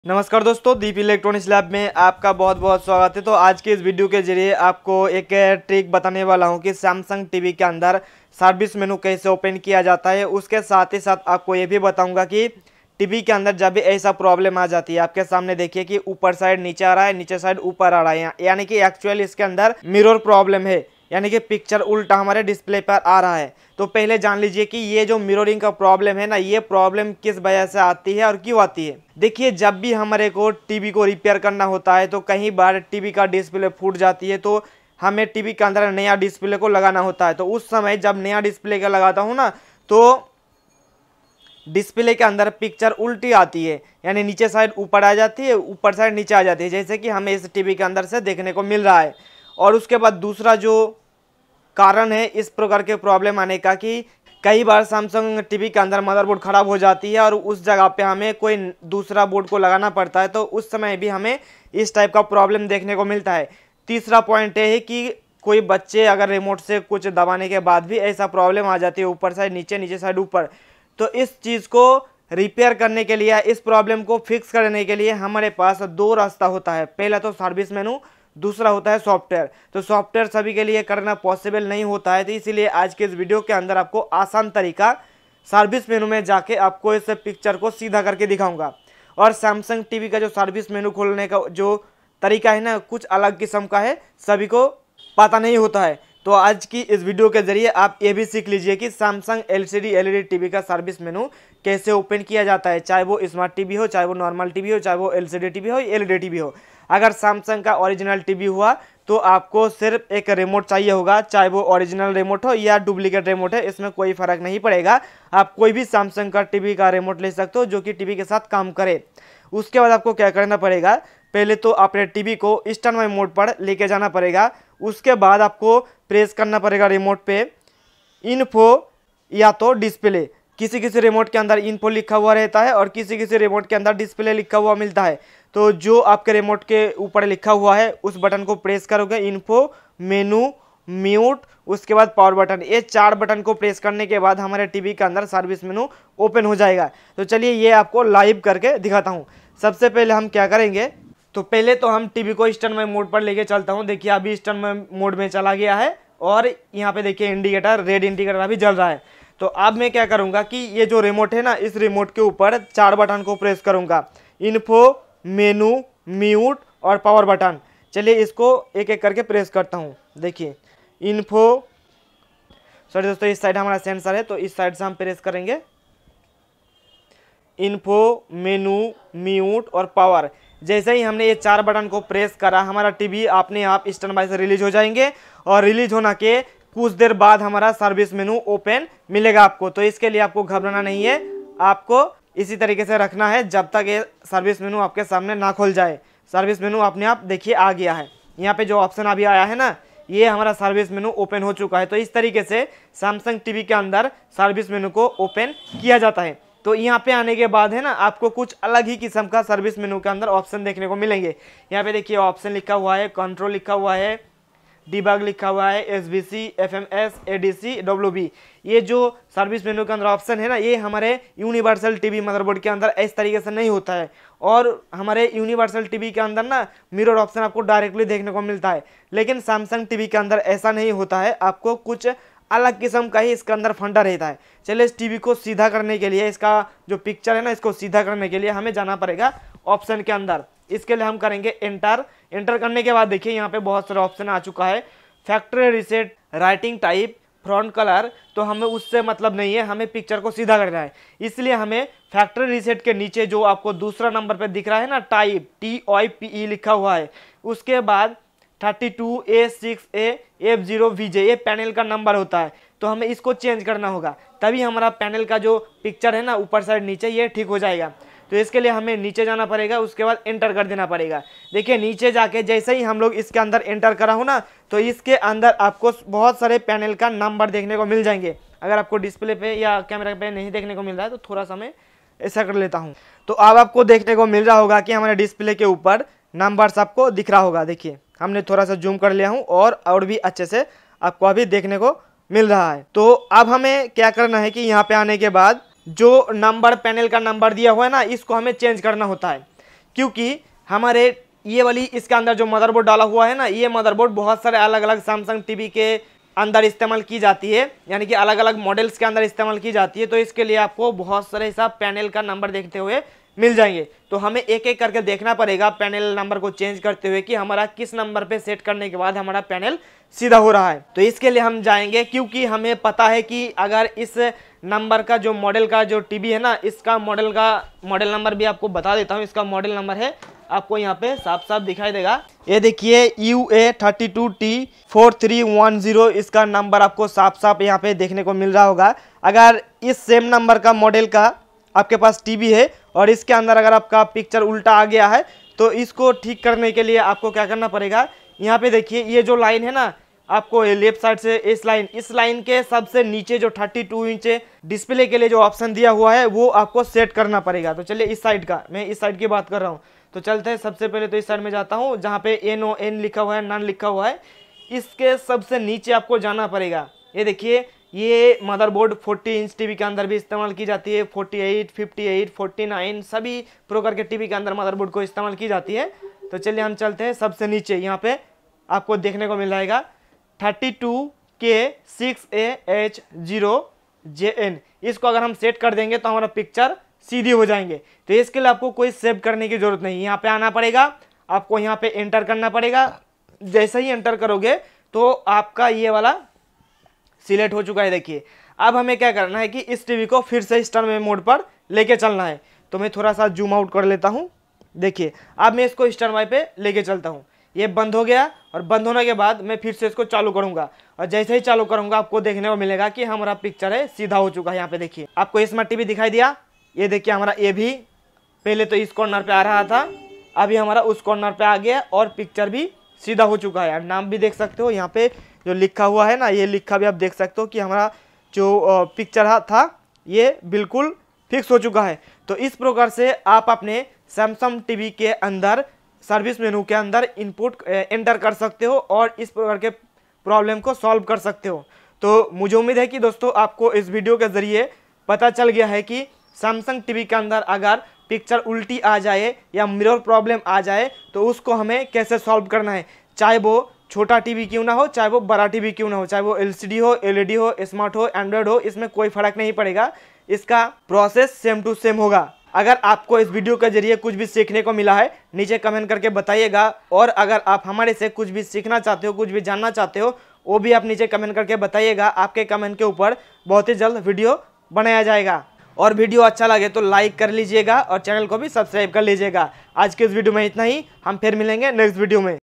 Samsung LED TV Mirror Problem Solving Tricks | All Samsung TV Service Menu Opening Tricks
In this video, we will show you some simple tricks to solve mirror problems on your Samsung LED TV. We'll cover topics such as checking your connections, adjusting picture settings, and other troubleshooting techniques that can help you fix the issue. Whether you're dealing with a distorted mirror image or other problems, these tips will help you get your TV back to normal in no time.